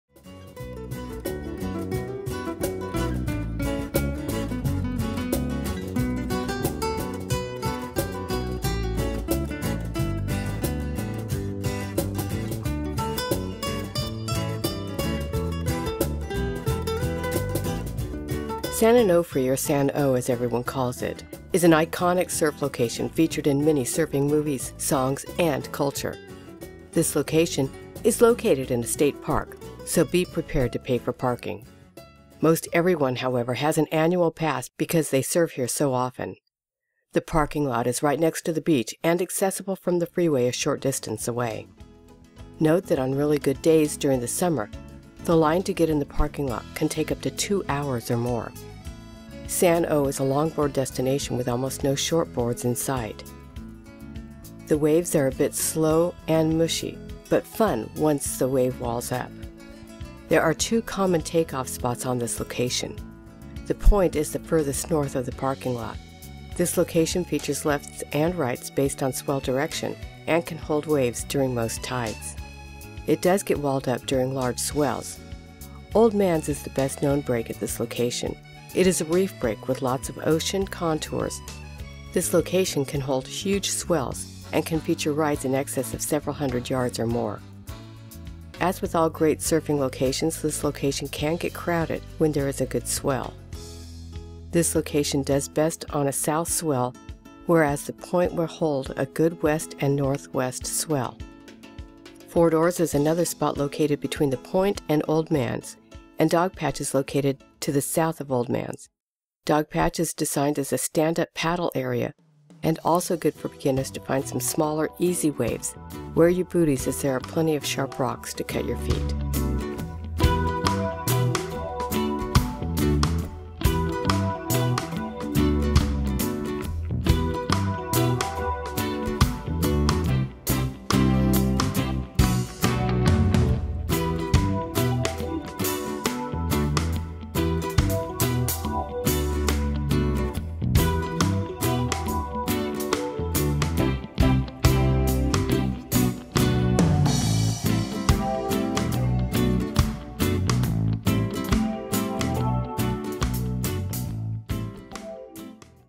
Surfing San Onofre
San Onofre or just San O as everyone calls it is a classic long board break in orange county. There are three main take off locations that feature long, slow waves perfect for long boards. This is a great family location where you will see all ages of surfers and all experience levels.